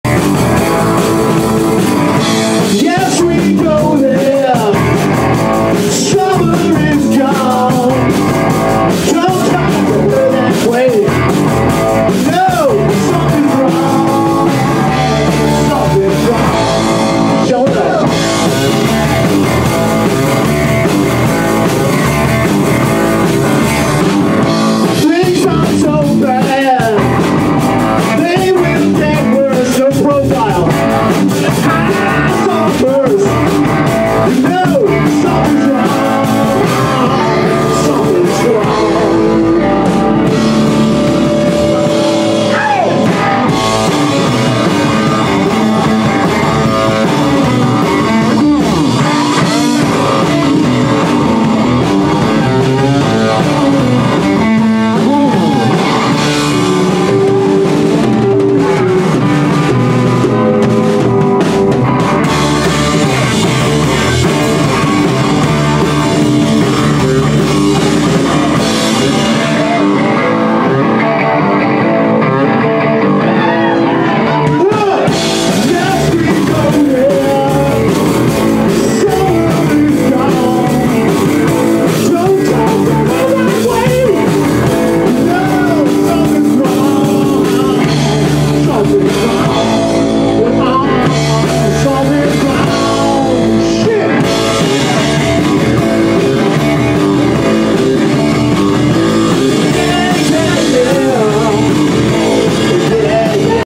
The Figgs @ Circle A 8-07-19
The Figgs played to a large crowd at Circle A 8-07-19. Winter Bear opened for them in a rare Milwaukee appearance.